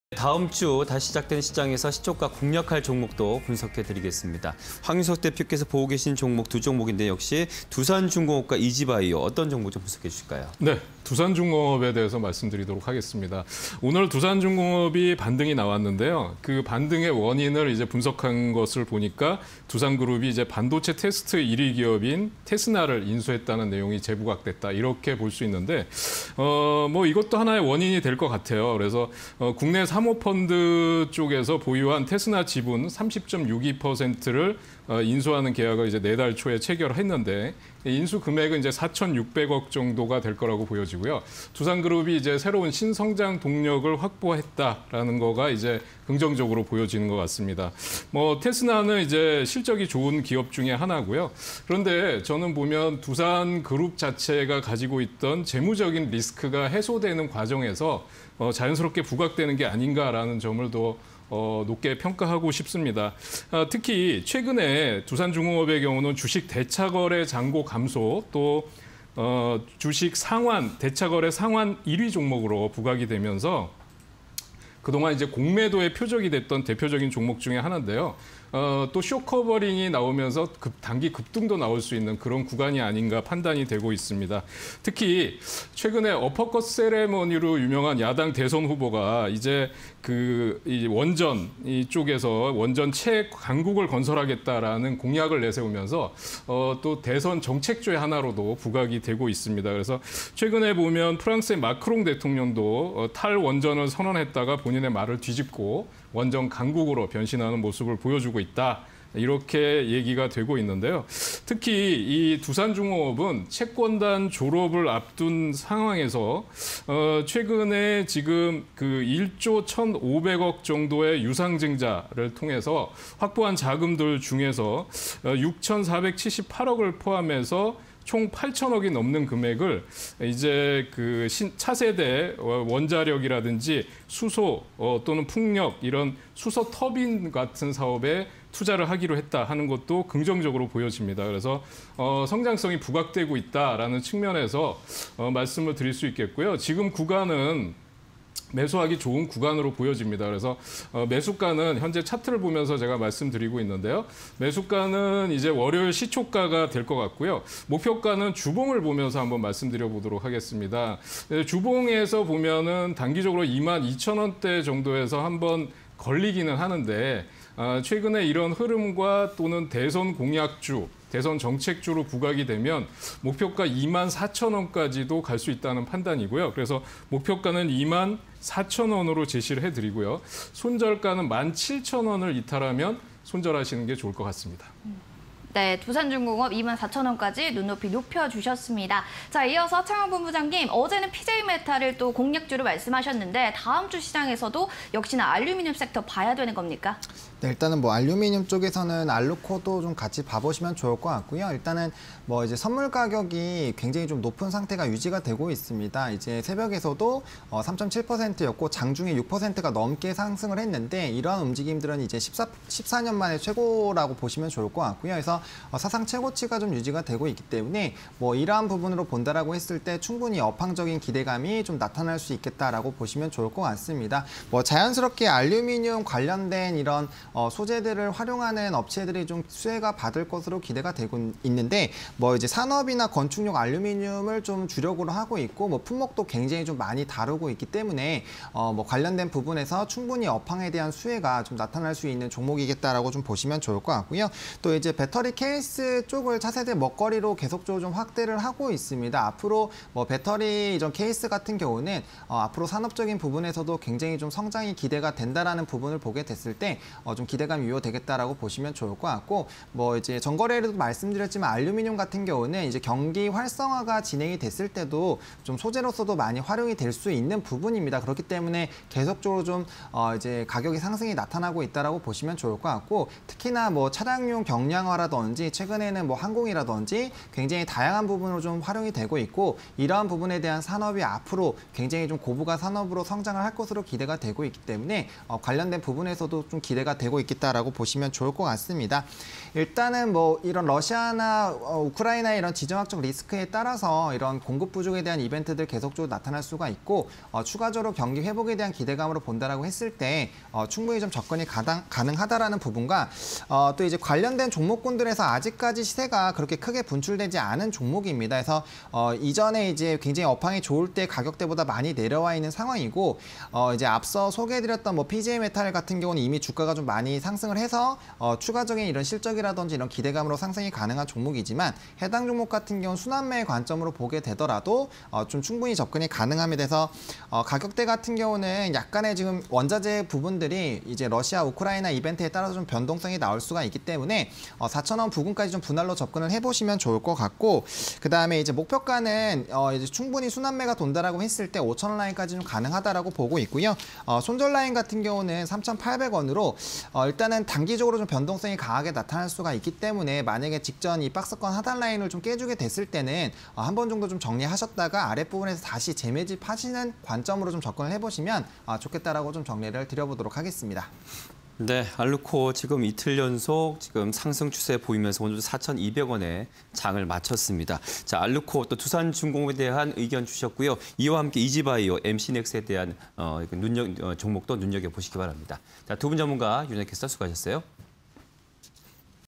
두산중공업, 알루코_사가지 (20220225)
두산중공업(034020)
- 반도체 테스트 1위 '테스나' 인수 재부각
- 원전 관련 공약에 정치테마주로 분류
- 수소터빈 등 신성장 사업에 투자

알루코(001780
- 산업ㆍ건축용 알루미늄 소재 가공 기업
- 알루미늄 가격 상승세 흐름
- 알루미늄ㆍ그린메탈 등 사용 수요 증가 기대